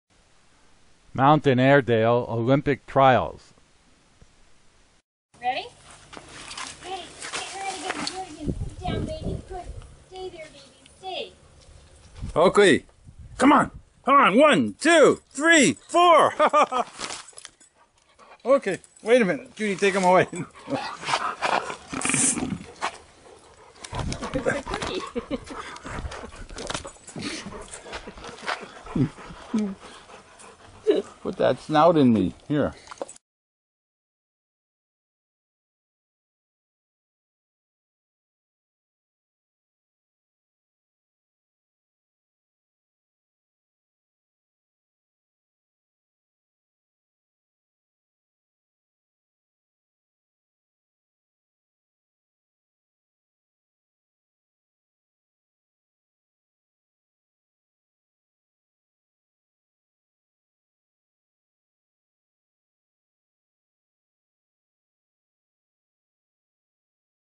Mountain Airedale Olympic Trials
Our Mountain Airedale, Oakley doing his 1st. generation set of Olympic hurdles. I a soon to be released new video, you'll see how we improved Oakley's hurdles by putting together a custom build, fully adjustable hurdle set.